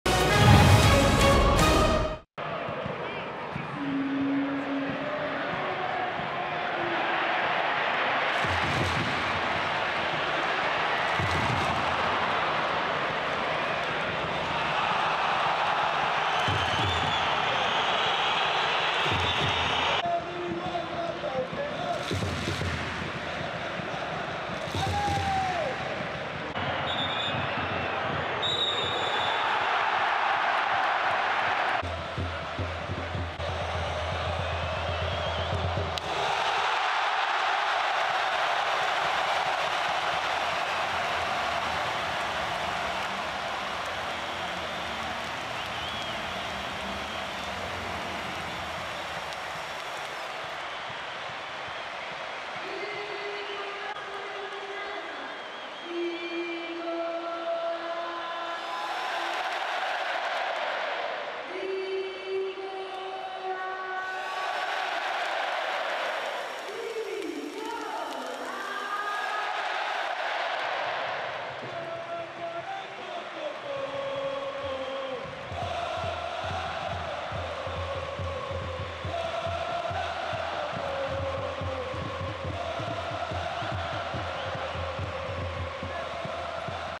But Nicolas PEPE (77' pen) / LOSC - Angers SCO (5-0) (LOSC-SCO)/ 2018-19
LOSC - Angers SCO ( 5-0) But de Nicolas PEPE (77' pen). Tous les buts de LOSC - Angers SCO en vidéo.

Ligue 1 Conforama - Saison 2018/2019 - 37ème journée Stade Pierre Mauroy - samedi 18 mai 2019

Buteurs : Jonathan IKONE (2' - LOSC) / Nicolas PEPE (14' - LOSC) / Jonathan BAMBA (70' - LOSC) / Nicolas PEPE (77' pen - LOSC) / Luiz ARAUJO (88' - LOSC)